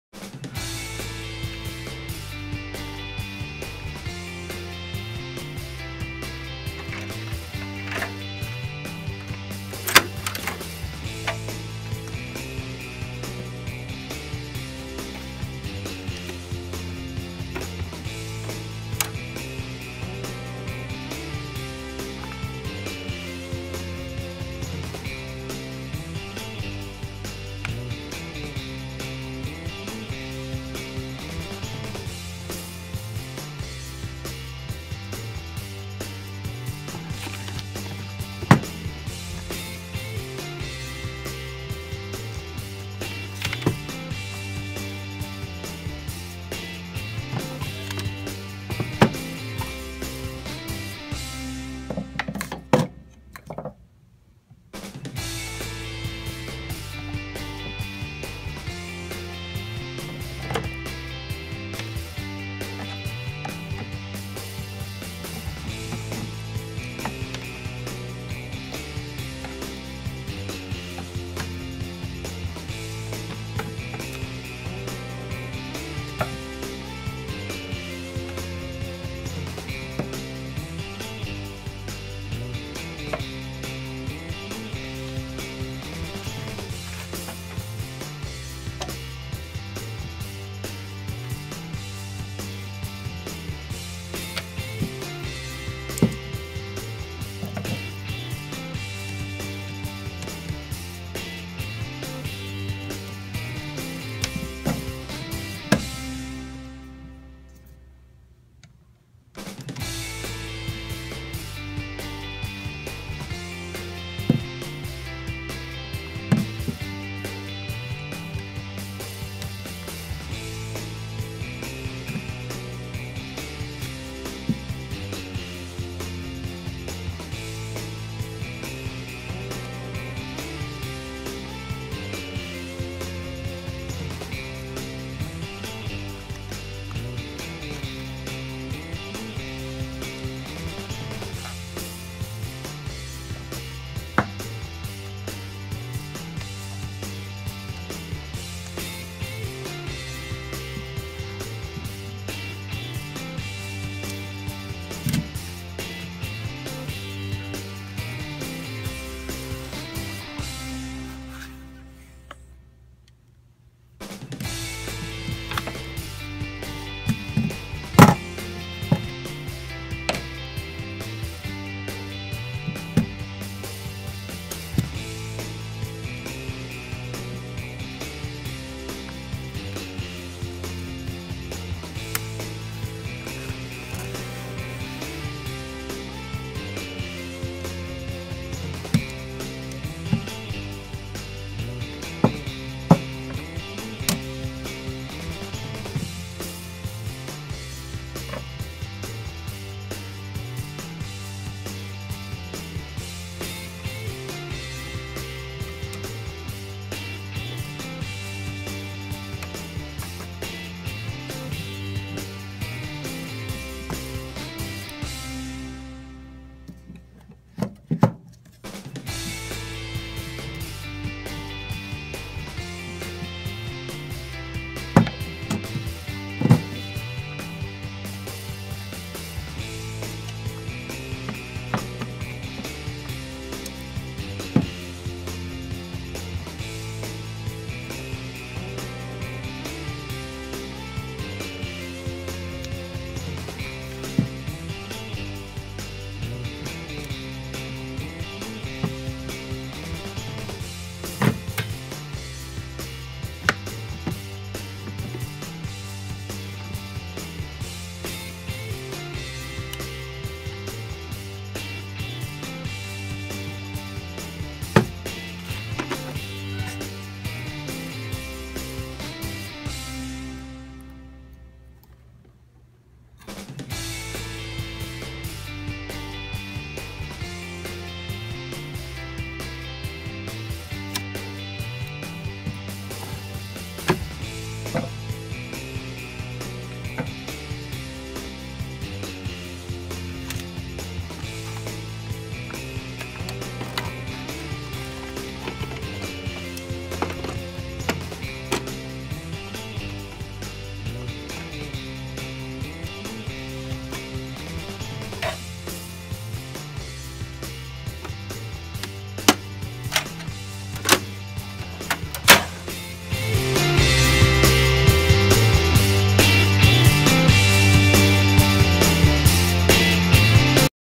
remove the transfer belt on Ricoh Aficio 2035, 3025, 3035, 3045, Copier
remove the transfer belt and cleaning assembly of a Ricoh Aficio 2035, 3025, 3035, 3045, Copier.